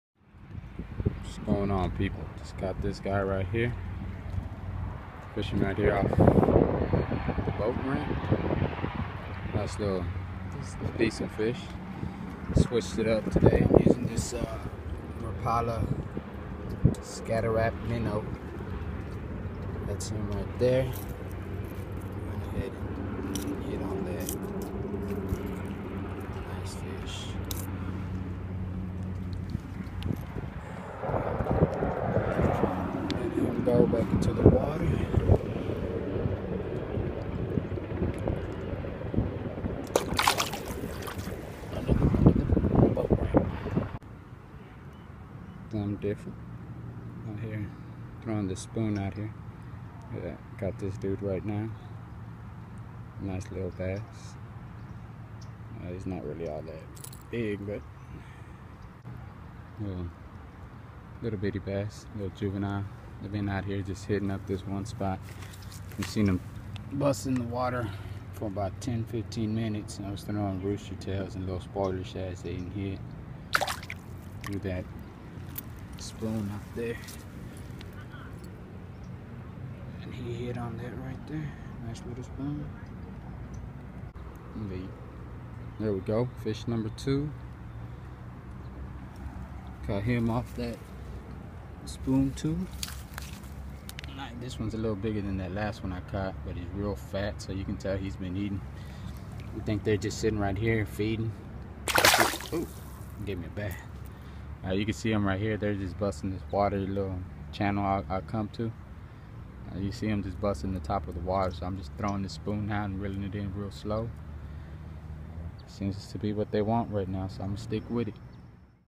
Spoon Feeding Bass
Caught a couple bass with a spoon today. Had fun making vid hope y'all like it.